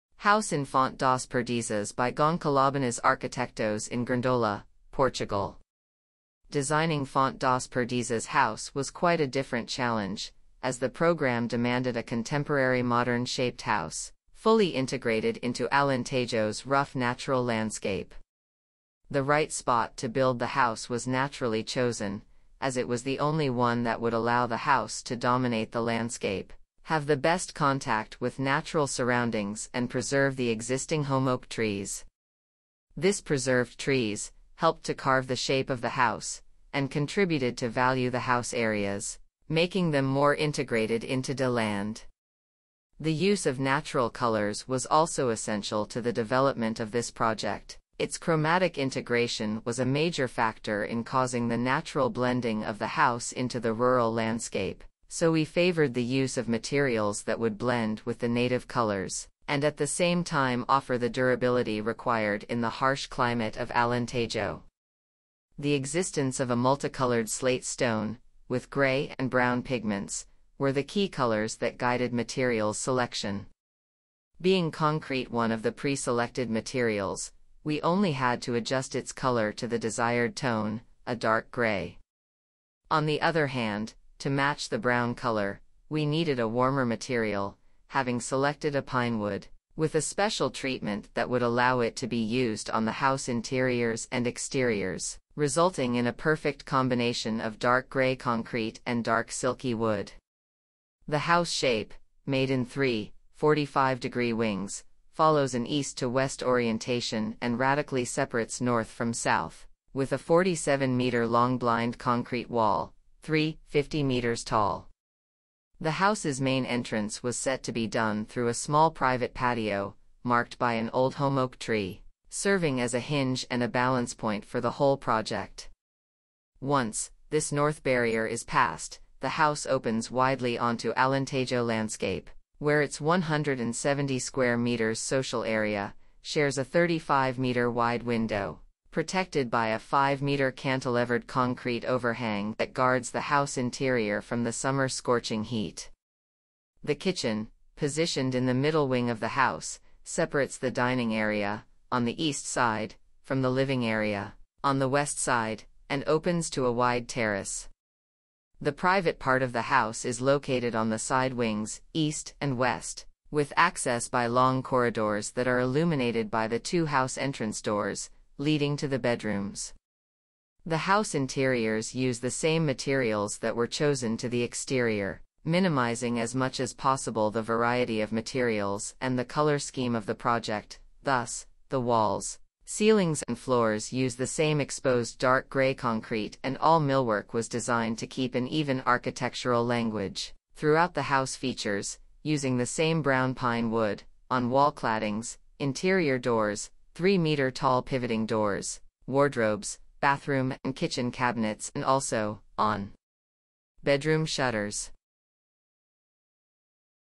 House in Fonte das Perdizes By gonçalobonniz arquitectos In GRÂNDOLA, PORTUGAL
Designing Fonte das Perdizes house was quite a different challenge, as the program demanded a contemporary modern shaped house, fully integrated into Alentejo’s rough natural landscape.
The right spot to build the house was naturally chosen, as it was the only one that would allow the house to dominate the landscape, have the best contact with natural surroundings and preserve the existing holm-oak trees. This preserved trees, helped to carve the shape of the house, and contributed to value   the house areas, making them more integrated into de land.

Archi Studio

Architects: gonçalobonniz arquitectos
Area: 500 m²
Year: 2021
Photographs:Geraldine Bruneel, José Campos
Manufacturers:  Kebony, CR Cerâmicas, OHM, Secil
Lead Architect: Gonçalo Bonniz
Project Team: Teresa Cabido
Structure: Elíptica - Engº Rui Pires
Installations: Sustentável - Engº Jorge Rosa
Landscape Design: Greengest - Arqtª Ana Videira
Decoration: Flores Textile Studio - Emma Pucci, Valentina Pilia
Construction Company: Matriz - Engº Filipe Queiroz
City: Grândola
Country: Portugal